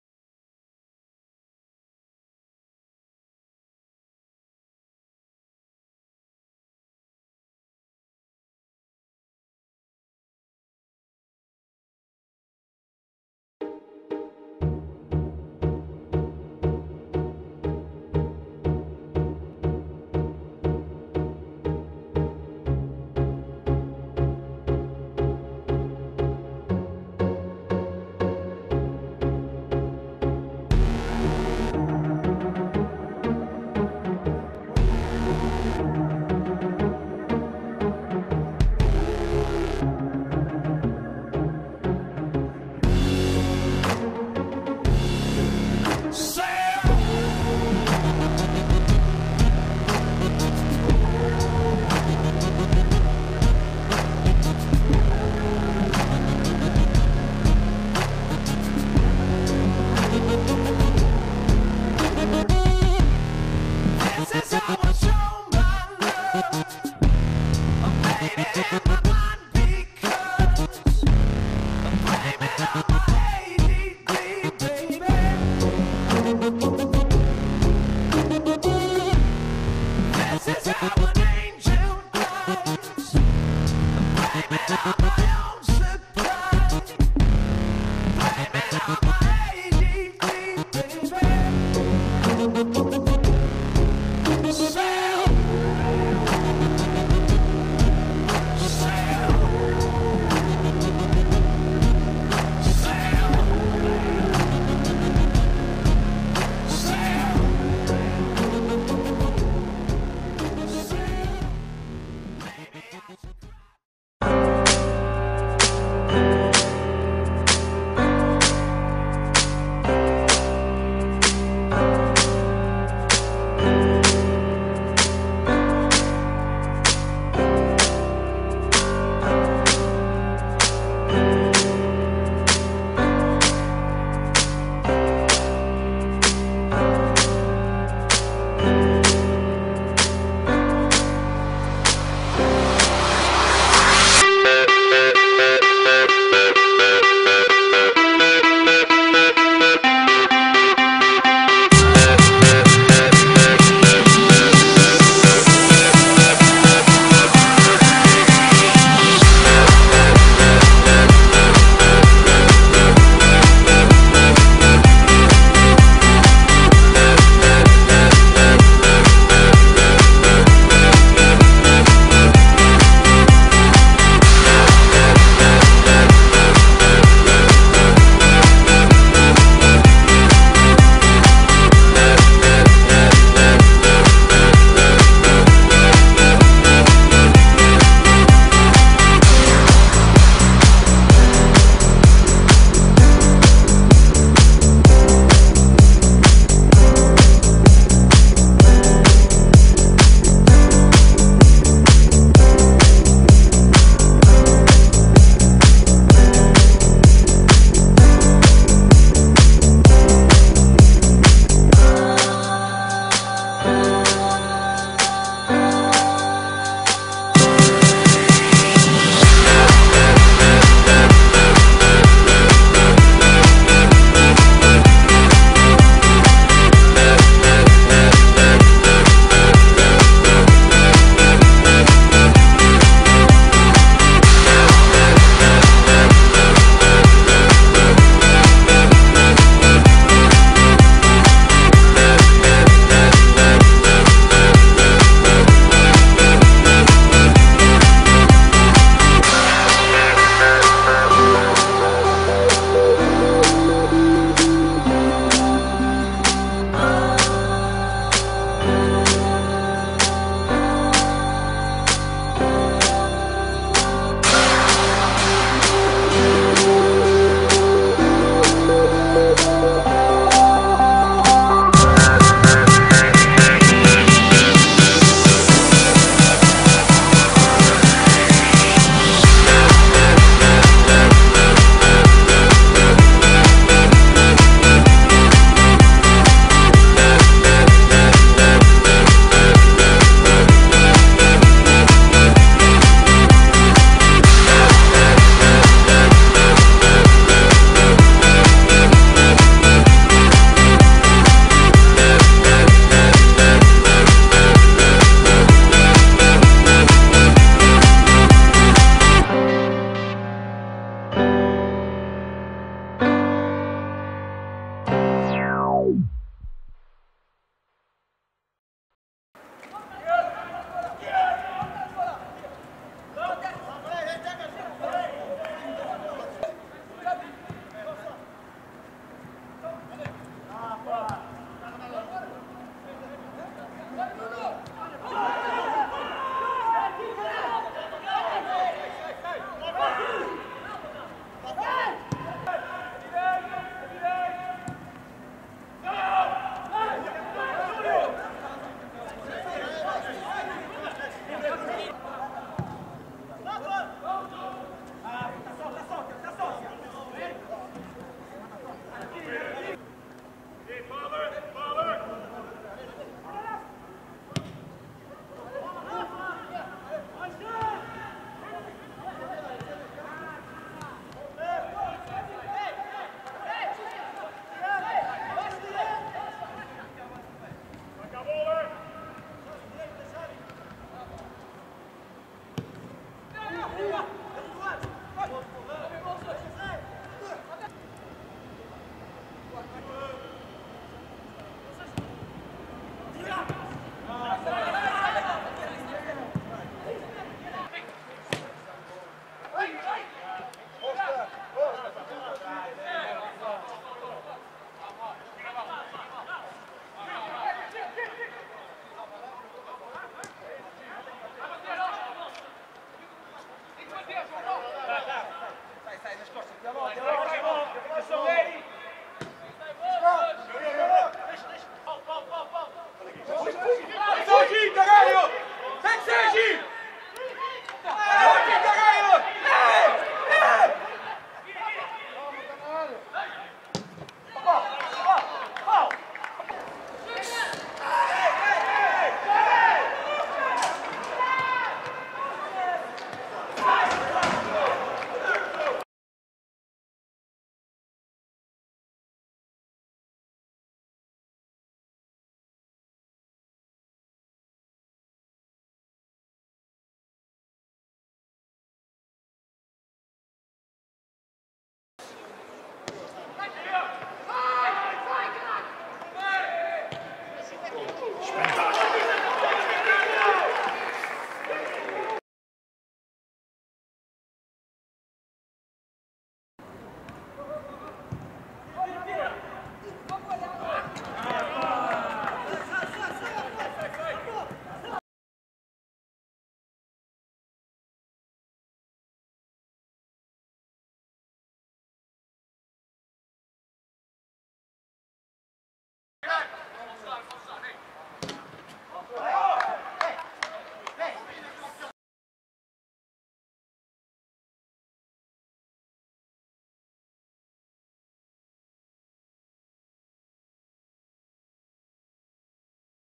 Goal-Keeper Rafael Alves 2011/12 - Junior
This is a small video of the goal keeper from Braga (Juniors) and Portuguese National Teams Under